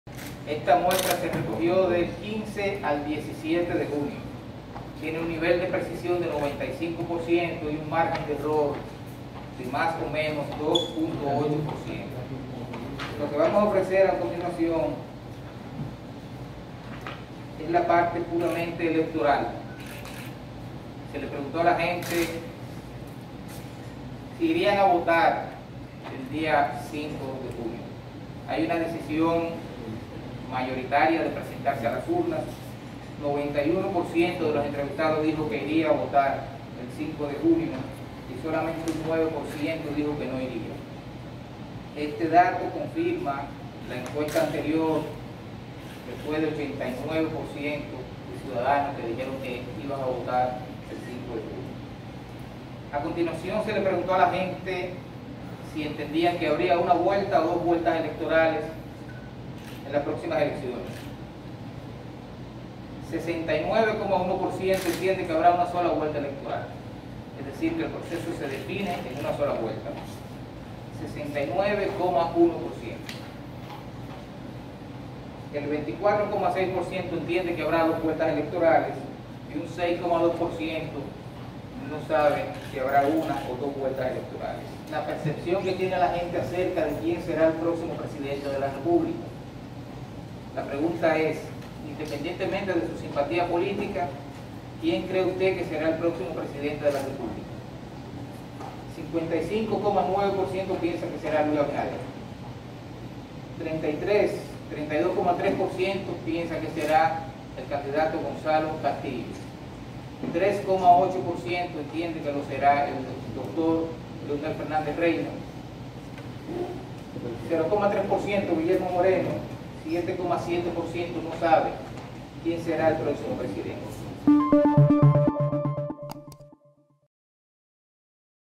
Abinader 55.8%, Gonzalo 33.5% y Leonel 7.0%, según Mercado y Cuantificaciones
El nuevo estudio presentado por la firma encuestadora Mercado y Cuantificaciones, le otorga un 55.8% a Luis Abinader, candidato presidencial del Partido Revolucionario Moderno (PRM); un 33.5% a Gonzalo Castillo, del Partido de la Liberación Dominicana (PLD); un 7.0% a Leonel Fernández, de la Fuerza del Pueblo (FP) y un 0.3% a Guillermo Moreno, de Alianza País (Alpaís).

La encuesta se realizó de forma presencial, del 15 al 17 del mes de junio con una muestra de 1,200 personas, en el territorio nacional, con un nivel de confianza de un 95% y un margen de error de un 2.8%, de acuerdo a Wilson Rodríguez, presidente de la firma encuestadora.